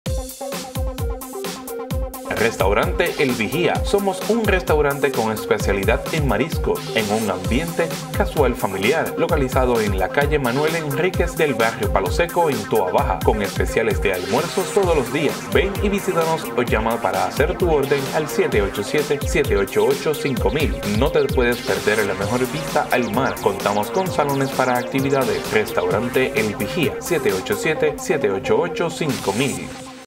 Restaurante El Vigia
"Nuestra especialidad son los mariscos."
Restaurante con especialidad en mariscos en un ambiente casual familiar. Localizado en la Calle Manuel Enríque del Barrio Palo Seco en Toa Baja.